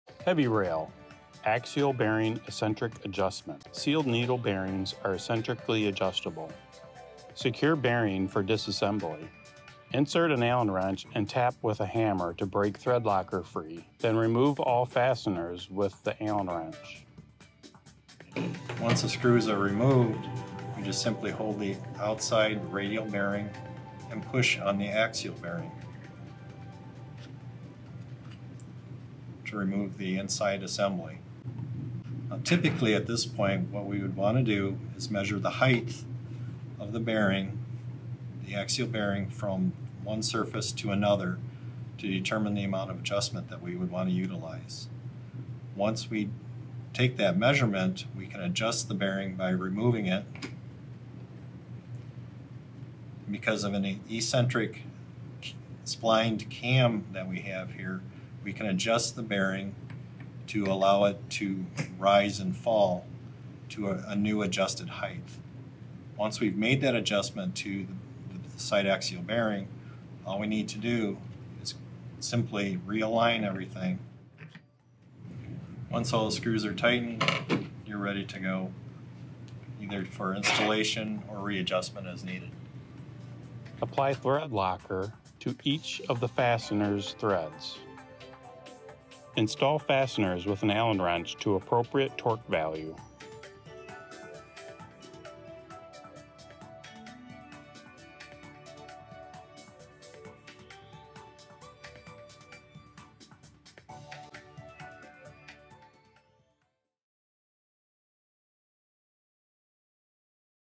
Hevi Rail Bearing Adjustment
Need adjustment assistance? Check out our updated how-to video on adjusting Hevi-Rail® combination bearings with Dave Walden, one of PBC Linear's Senior Application Engineers.